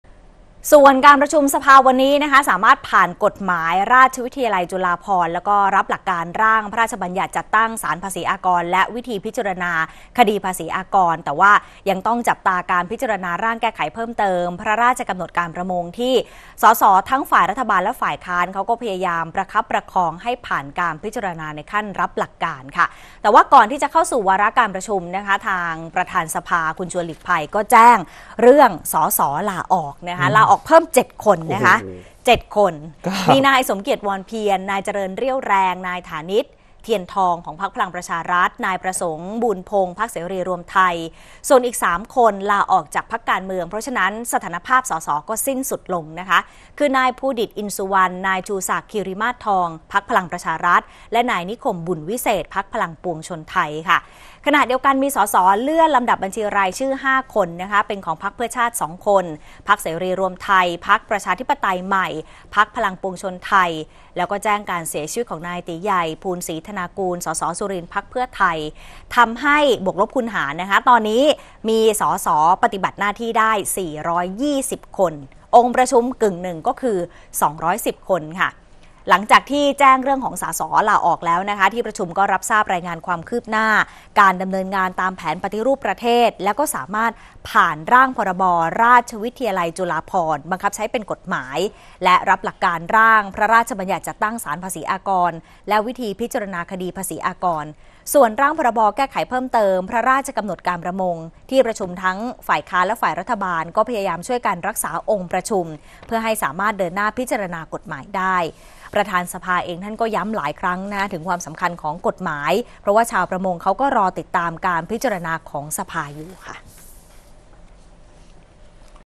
ลุ้นสภา รับหลักการ พ.ร.บ.ประมง ข่าวค่ำ วันที่ 9 กุมภาพันธ์ 2566
การประชุมสภาฯ วันนี้ สามารถผ่านกฎหมายราชวิทยาลัยจุฬาภรณ์ พร้อมรับหลักการร่างพระราชบัญญัติจัดตั้งศาลภาษีอากรและวิธีพิจารณาคดีภาษีอากร แต่ยังต้องจับตาการพิจารณาร่างแก้ไขเพิ่มเติมพระราชกำหนดการประมง ที่ ส.ส.ฝ่ายรัฐบาลและฝ่ายค้าน พยายามประคับประคองให้ผ่านการพิจารณาขั้นรับหลักการ